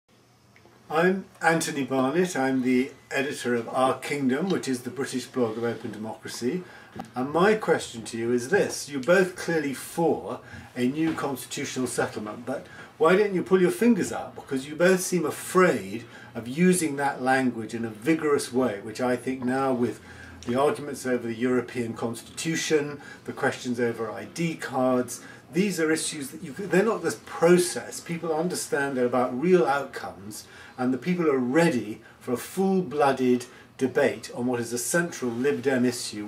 Anthony Barnett asks about constitutional change
Anthony Barnett, the editor of Our Kingdom, asks the Liberal Democrat leadership candidates why they aren't doing more to campaign on constitutional change.